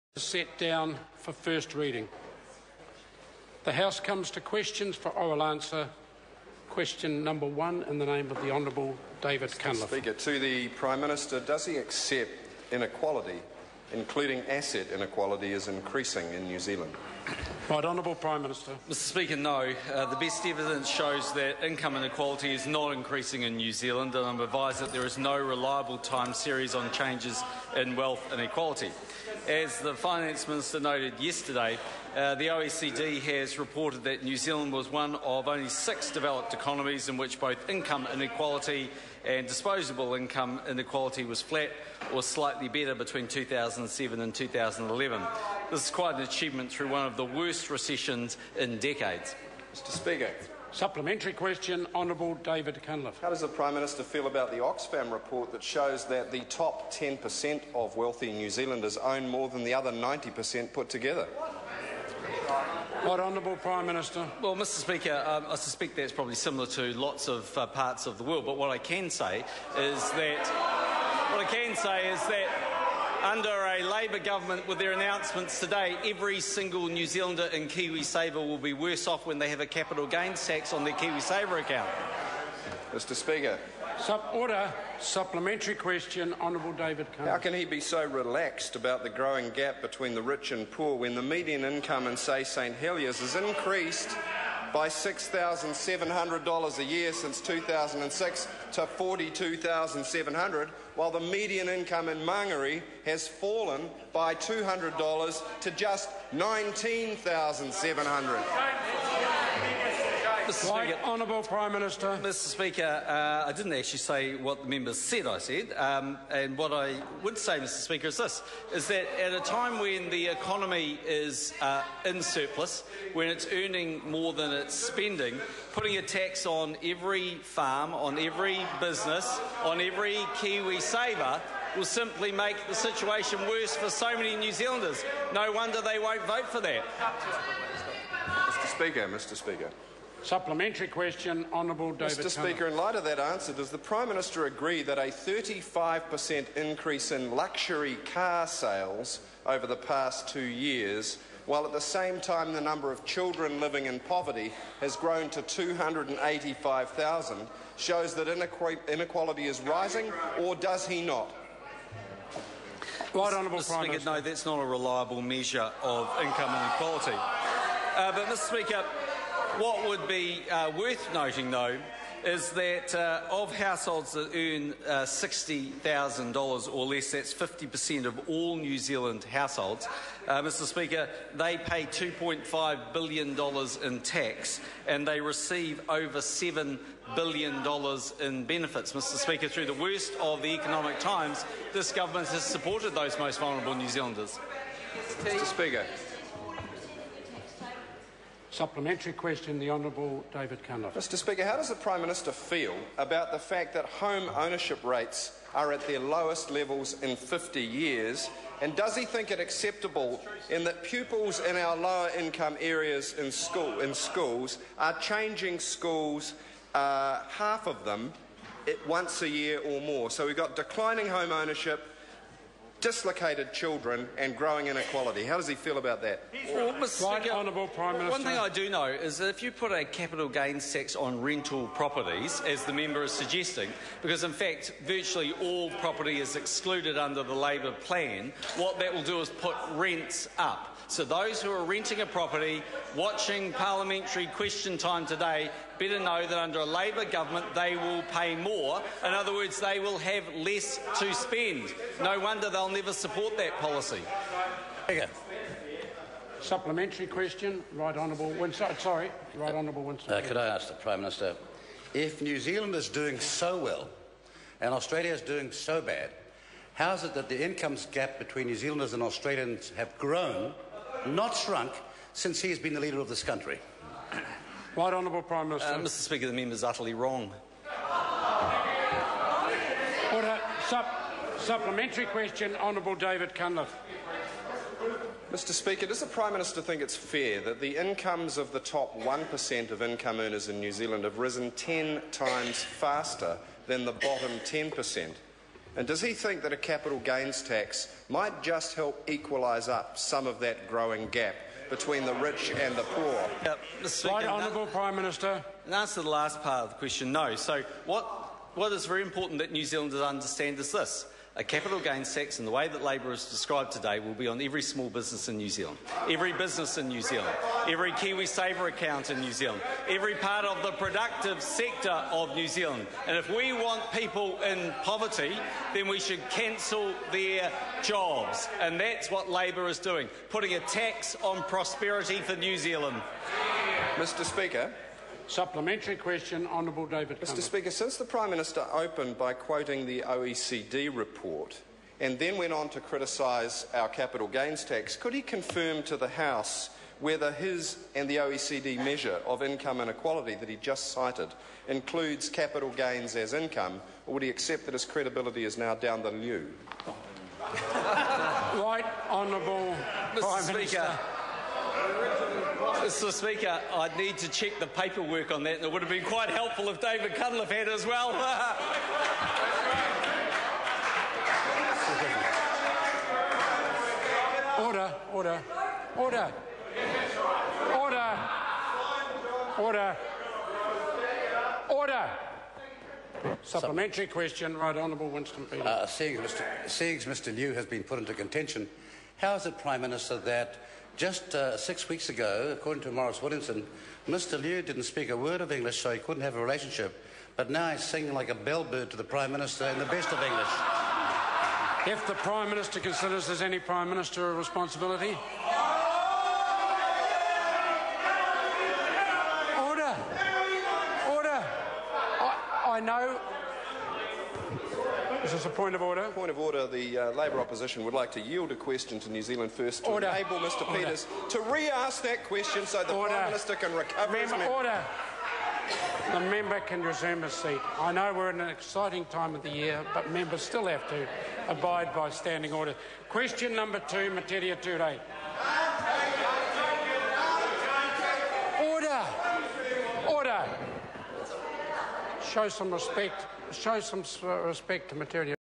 25.06.14 - Question 1: Hon David Cunliffe to the Prime Minister
Does he accept inequality, including asset inequality, is increasing in New Zealand?